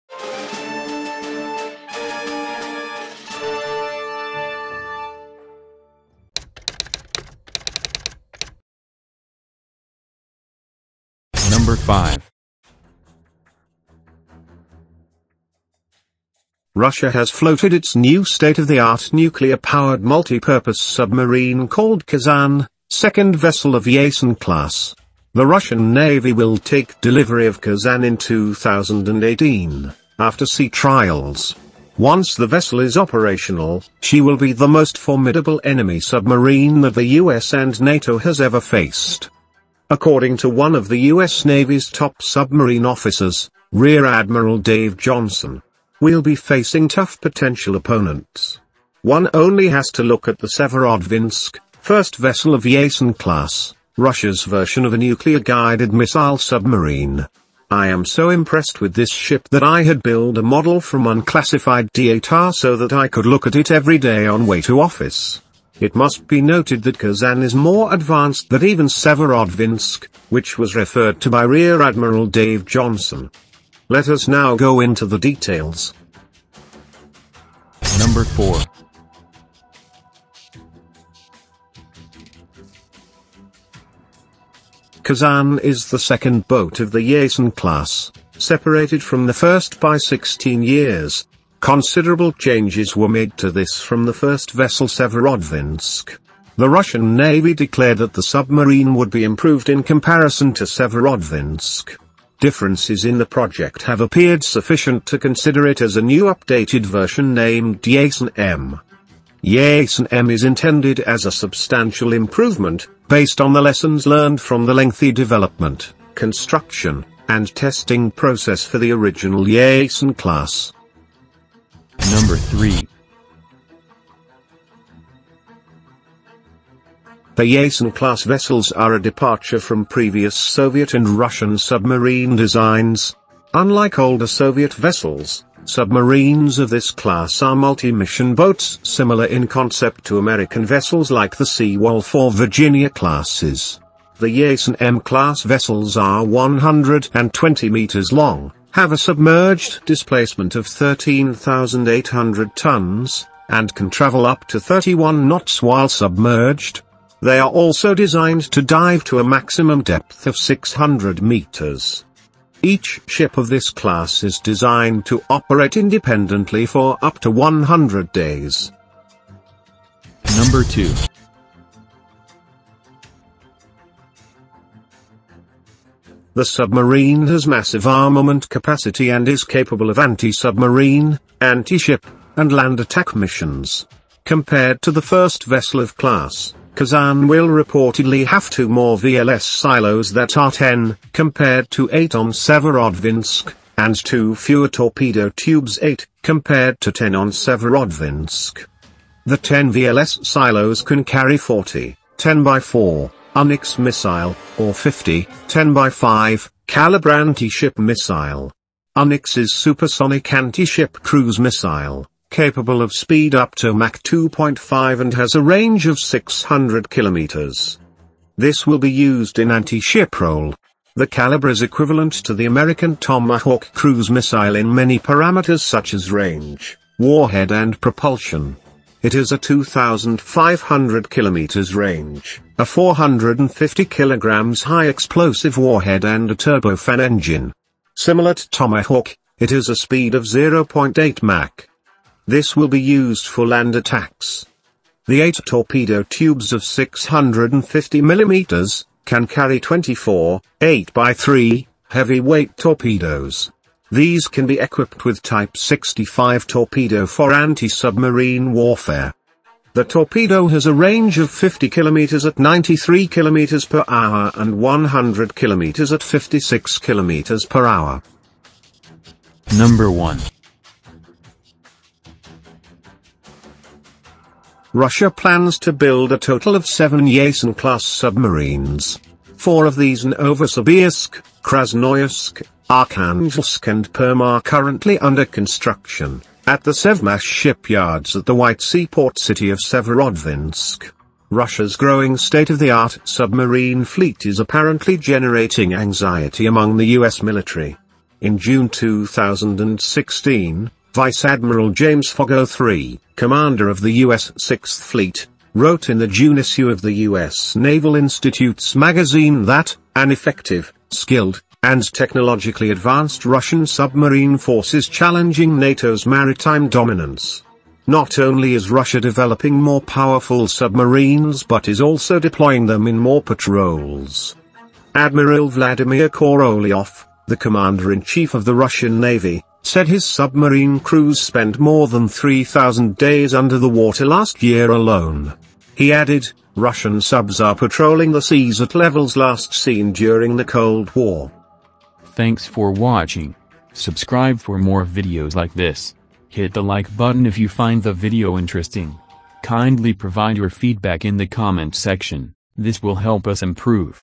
RUSSIA READYING KAZAN, MOST POWERFUL ATTACK SUBMARINE: TOP 5 FACTS
INTRODUCTION:
Russia has floated its new state-of-the-art nuclear-powered multipurpose submarine called Kazan, second vessel of Yasen class.
The Russian Navy will take delivery of Kazan in 2018, after sea trials. Once the vessel is operational, she will be the most formidable enemy submarine that the U.S. & NATO has ever faced.
According to one of the U.S. Navy’s top submarine officers, Rear Admiral Dave Johnson.
"We’ll be facing tough potential opponents. One only has to look at the Severodvinsk, [first vessel of Yasen class], Russia’s version of a nuclear guided missile submarine (SSGN). I am so impressed with this ship that I had build a model from unclassified da ta so that he could look at it every day on his way to his office”.
It must be noted that Kazan is more advanced that even Severodvinsk, which was referred to by Rear Admiral Dave Johnson.
Let us now go into the details.

DEVELOPMENT

Kazan is the second boat of the Yasen class, separated from the first by 16 years (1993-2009). Considerable changes were made to this from the first vessel Severodvinsk.
The Russian navy declared that the submarine would be improved in comparison to Severodvinsk.
Differences in the project have appeared sufficient to consider it as a new updated version named Yasen-M.
Yasen-M is intended as a substantial improvement, based on the lessons learned from the lengthy development, construction, and testing process for the original Yasen class.

DESIGN:
The Yasen class vessels are a departure from previous Soviet and Russian submarine designs. Unlike older Soviet vessels, submarines of this class are multi-mission boats similar in concept to American vessels like the Seawolf or Virginia classes.
The Yasen-M class vessels are 120 meters long, have a submerged displacement of 13800 tons, and can travel up to 31 knots (57 kph) while submerged. They are also designed to dive to a maximum depth of 600 meters.
Each ship of this class is designed to operate independently for up to 100 days.

ARMAMENT:
The submarine has massive armament capacity and is capable of anti-submarine, anti-ship, and land-attack missions.
Compared to the first vessel of-class, Kazan will reportedly have 2 more VLS silos that are 10, compared to 8 on Severodvinsk, and 2 fewer torpedo tubes 8, compared to 10 on Severodvinsk.
The 10 VLS silos can carry 40 [ 10 x 4 ] Oniks missile, or 50 [ 10 x 5 ] Kalibr anti-ship missile.
Oniks is supersonic anti-ship cruise missile, capable of speed upto Mach 2.5 and has a range of 600 km. This will be used in Anti Ship role.
The Kalibr is equivalent to the American Tomahawk cruise missile in many parameters such as range, warhead and propulsion.
It has a 2500 km range, a 450 kg (1000 pound) high-explosive warhead and a turbofan engine. Similar to Tomahawk, it has a speed of 0.8 mach. This will be used for Land Attacks.

The 8 torpedo tubes of 650mm, can carry 24 [ 8 x 3 ]  heavy weight torpedoes.  These can be equipped with Type 65 torpedo for Anti Submarine warfare. The torpedo has a range of 50 km at 93 km/h and 100 km at 56 km/h.

CONCLUSION:

Russia plans to build a total of 7 Yasen class submarines. 4 of these Novosibirsk, Krasnoyarsk, Arkhangelsk and Perm are currently under construction, at the Sevmash shipyards at the White Sea port city of Severodvinsk.

Russia’s growing state-of-the-art submarine fleet is apparently generating anxiety among the US military. In June 2016, Vice Admiral James Foggo 3, commander of the US 6th Fleet, wrote in the June issue of the US Naval Institute’s magazine that, “an effective, skilled, and technologically advanced Russian submarine force is challenging” NATO’s maritime dominance.

Not only is Russia developing more powerful submarines but is also deploying them in more patrols.
Admiral Vladimir Korolyov, the commander-in-chief of the Russian navy, said his submarine crews spent more than 3,000 days under the water last year alone.

He added, Russian subs are patrolling the seas at levels last seen during the Cold War.